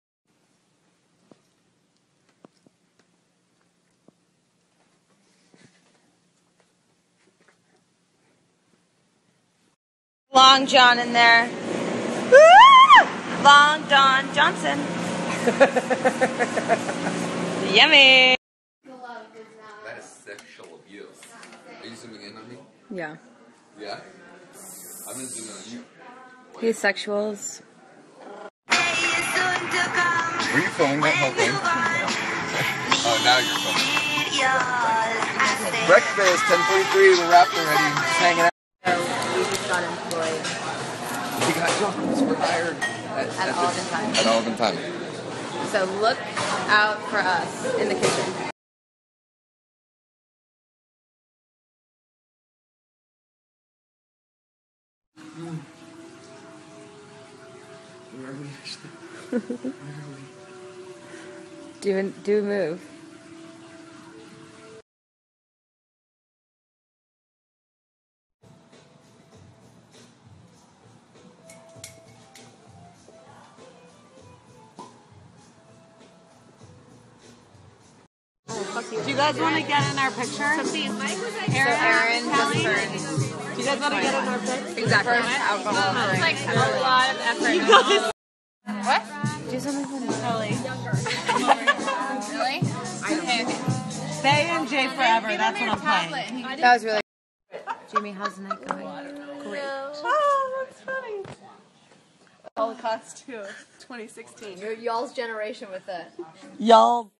Ashley Benson | August 26th 2015 | FULL SNAPCHAT STORY (featuring Tyler Blackburn)
Snapchats
Celebrity Snapchats
Snapchat Compilations
Vlogs
Celebrity Vlogs